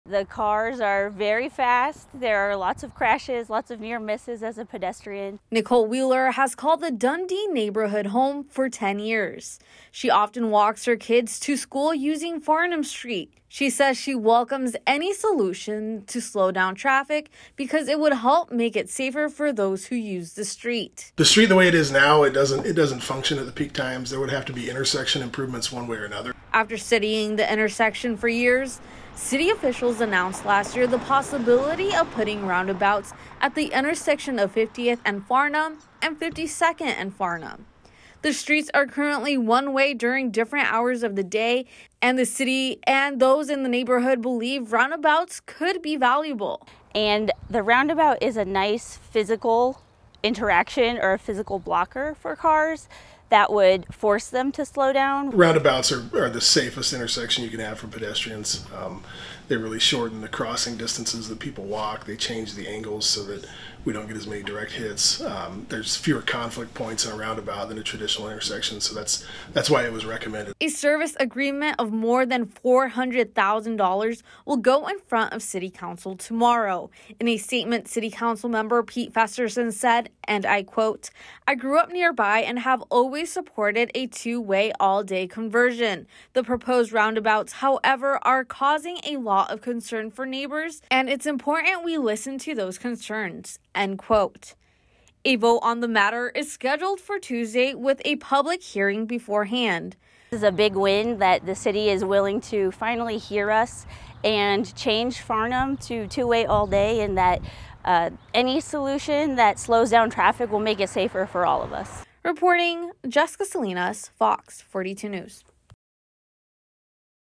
Omaha city council gearing up for public hearing and vote on stretch of Farnam Street
After studying the intersection for years, city officials came to the determination last year that a two-way street all day plus roundabouts in the area could be a good solution. On Tuesday, council members are scheduled to vote on a hiring firm for the design phase of that project.